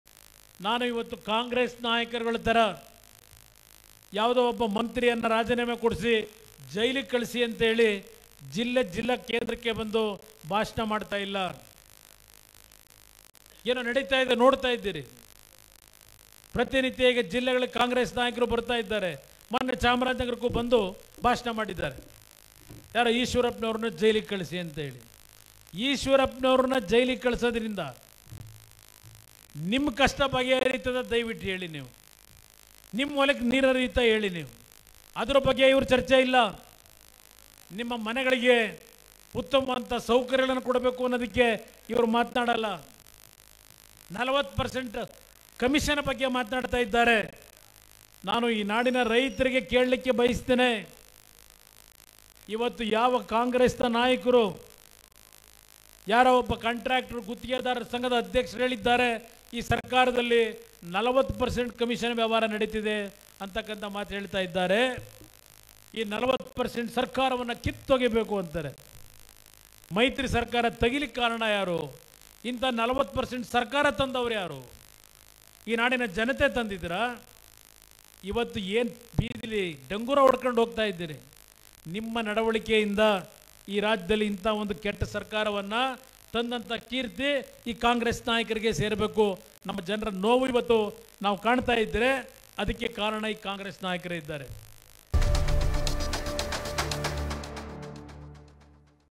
HD Kumarswamy : ಮೈತ್ರಿ ಸರ್ಕಾರವನ್ನು ಬೀಳಿಸಿ ಬಿಜೆಪಿ ಸರ್ಕಾರ ತಂದಿದ್ದೆ ಕಾಂಗ್ರೆಸ್!
HD Kumarswamy : ಕಾಂಗ್ರೆಸ್‌ನೋರ ತರಹ ಯಾವುದೋ ಮಂತ್ರಿಯನ್ನು ಅರೆಸ್ಟ್ ಮಾಡಿ ಎಂದು ಬಂದಿಲ್ಲ.  ಮೈತ್ರಿ ಸರ್ಕಾರವನ್ನು ಬೀಳಿಸಿ ಬಿಜೆಪಿ ಸರ್ಕಾರ ತಂದಿದ್ದೆ ಕಾಂಗ್ರೆಸ್!